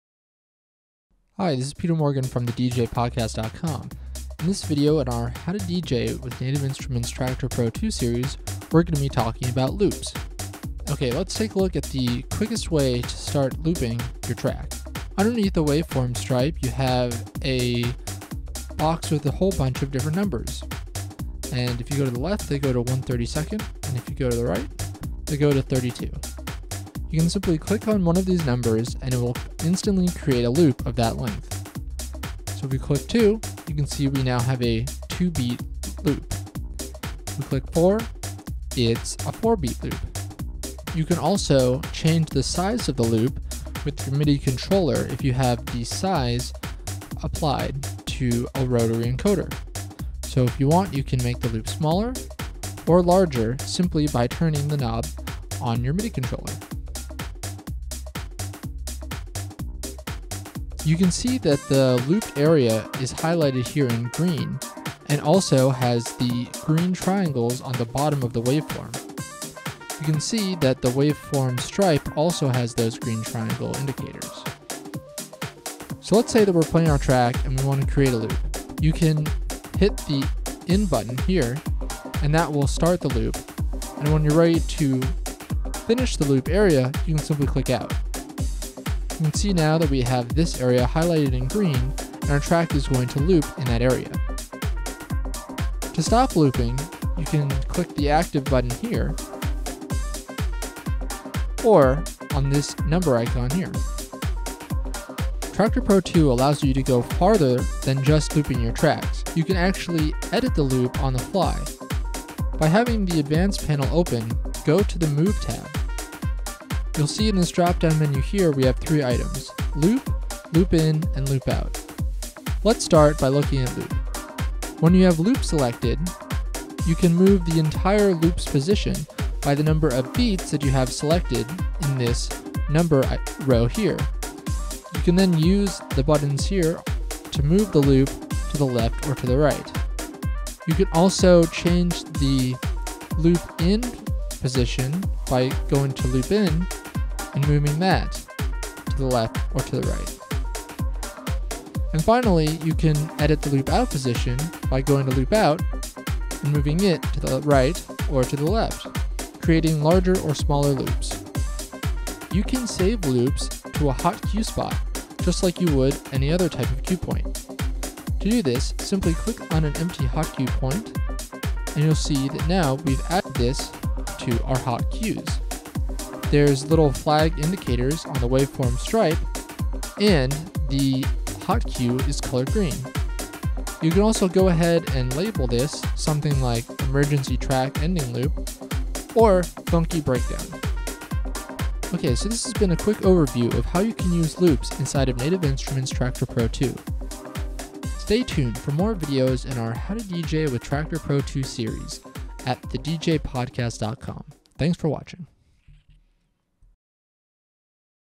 How to DJ with Traktor Pro 2: Part 7 - Get Loopy with Loops
This video tutorial is all about using loops in Native Instruments Traktor Pro 2. Learn how to loop sections of your tracks, edit the loop's size and position, and save your loop as a hot cue.